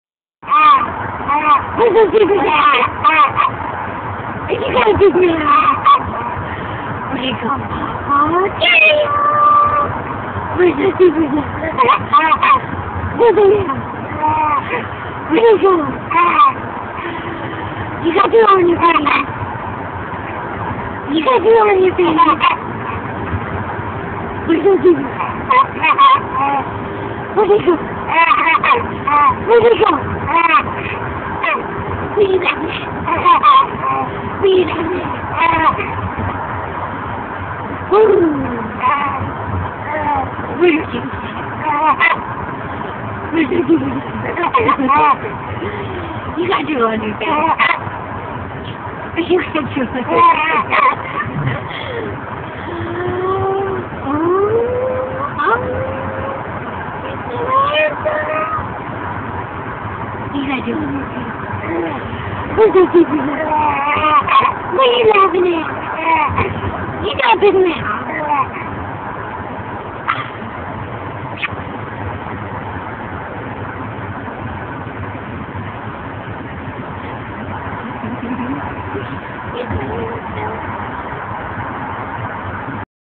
Baby Owen laughing in the truck!!
Owen in the car with his mommy making him laugh!! He's such a happy baby!!